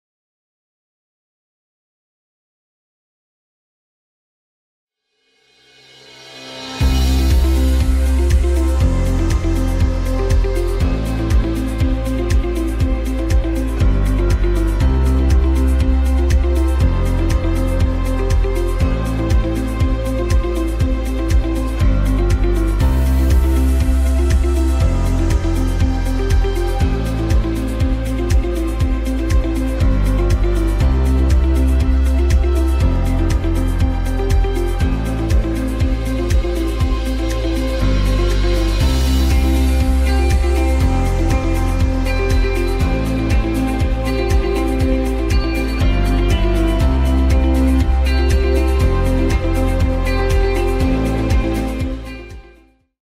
Meet Dr. Olusegun Mimiko
The Former Governor of Ondo State, Dr. Olusegun Mimiko will be our distinguished Guest Speaker at the Public Lecture of the 33rd IFUMSA Health, July 12. Join us next week Thursday in our advocacy for a safe motherhood at the Main Auditorium, OAUTHC!